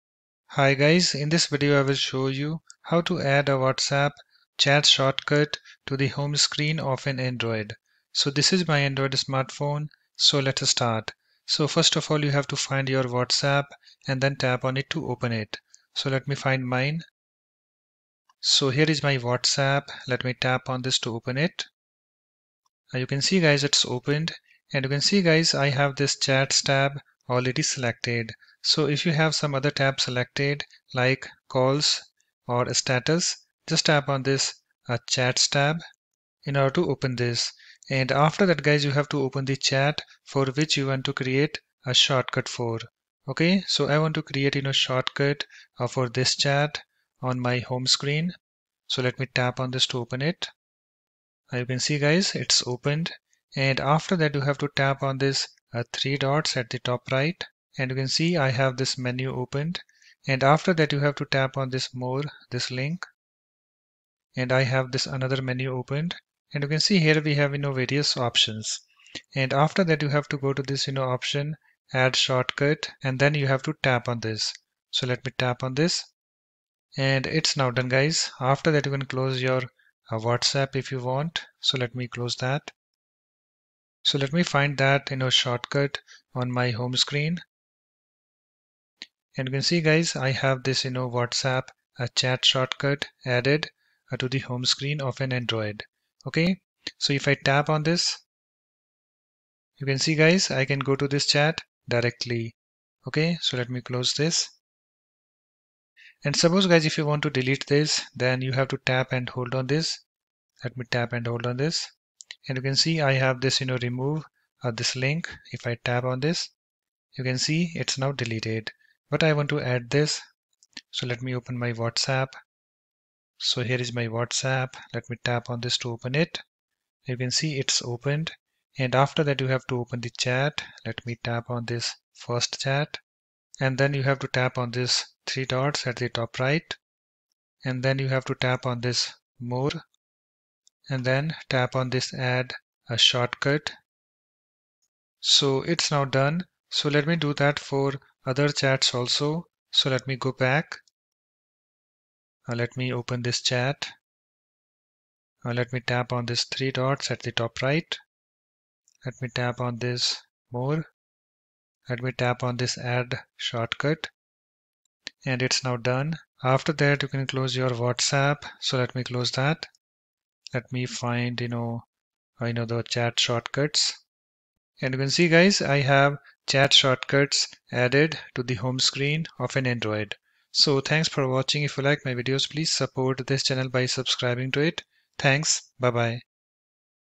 How to Add a WhatsApp Chat Shortcut to the Home Screen of an Android?
Hi guys, In this Video, I will show you 'How to Add a WhatsApp Chat Shortcut to the Home Screen of an Android.'